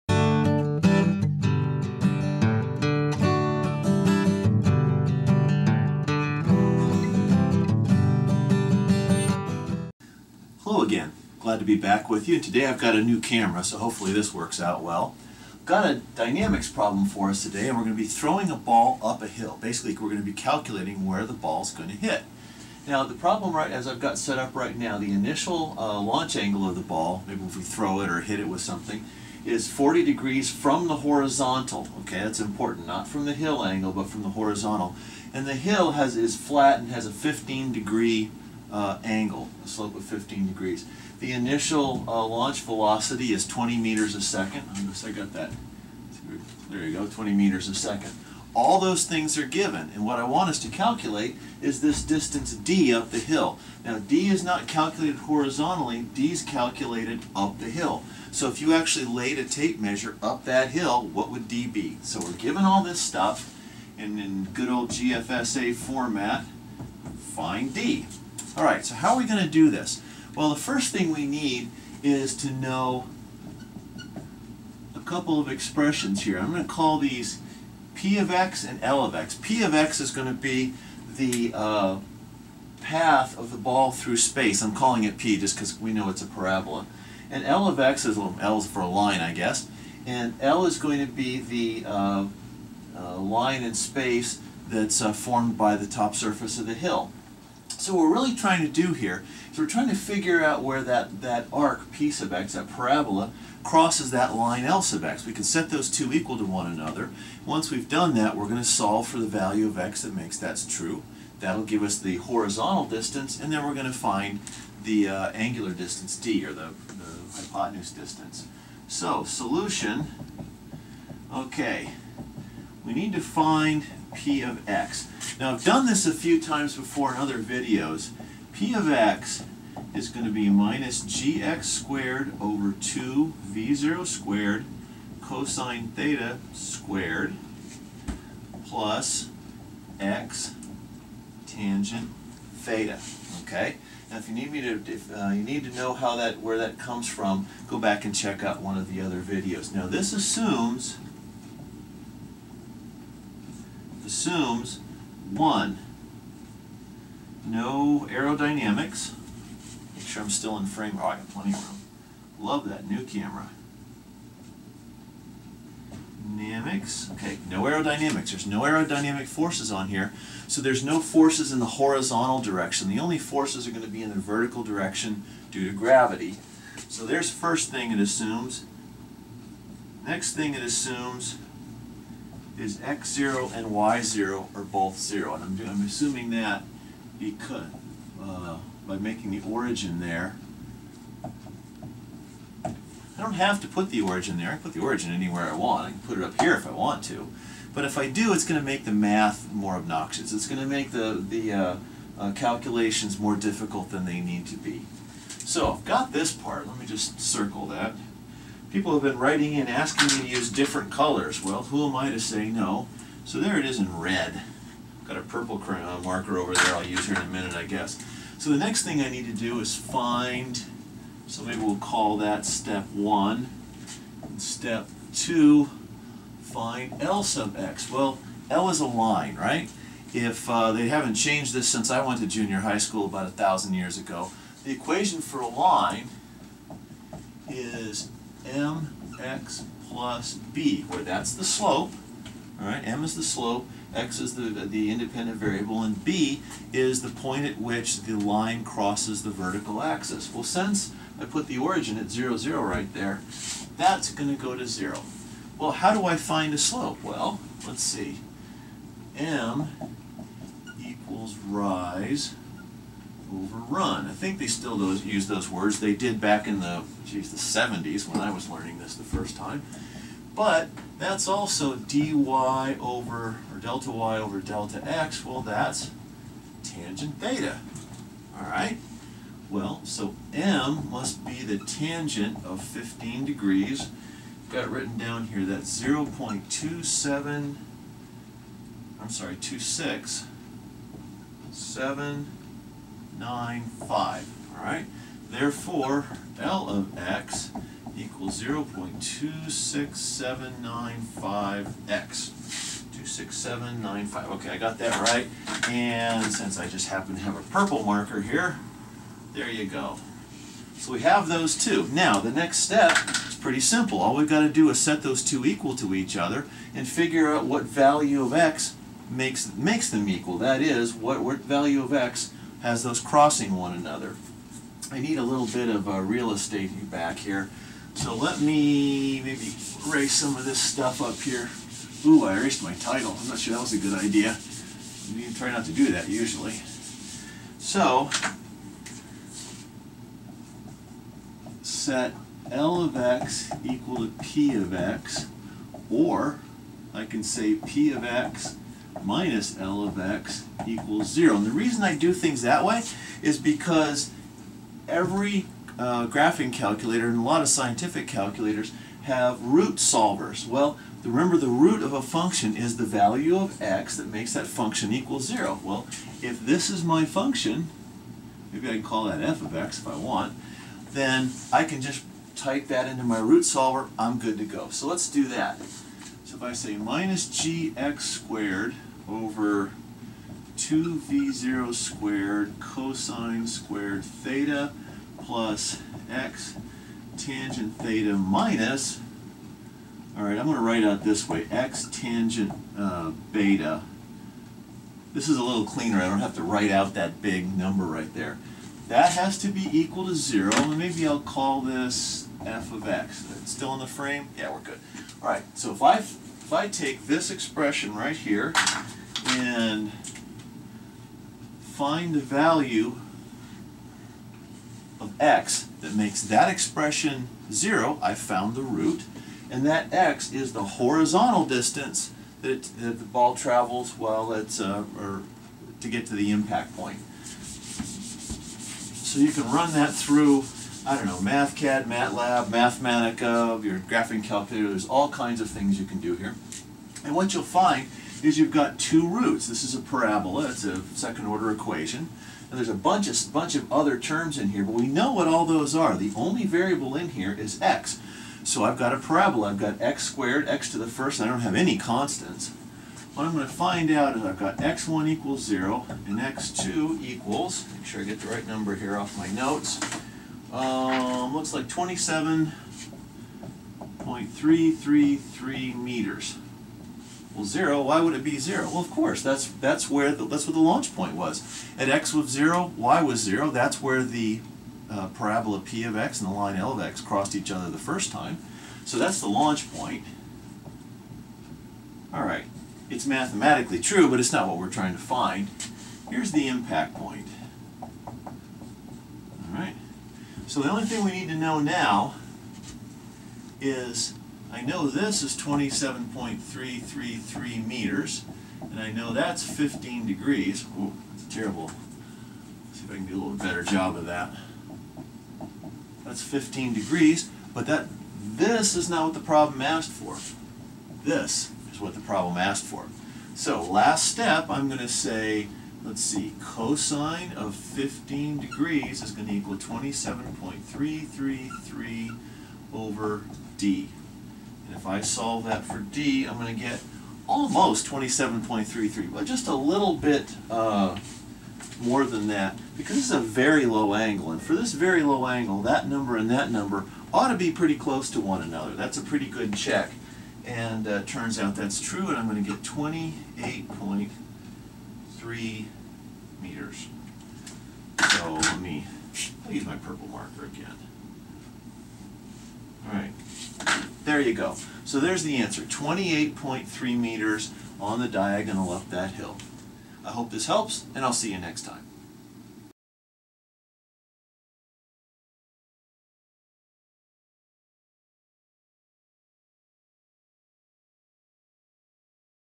Throwing a Ball Uphill Brain Waves
We're used to solving dynamics problems in which the projectile travels over a horizontal surface.  What happens when that's not true, like if a ball is thrown up a hill?  This problem only requires a small change to the process and isn't as difficult as you might think.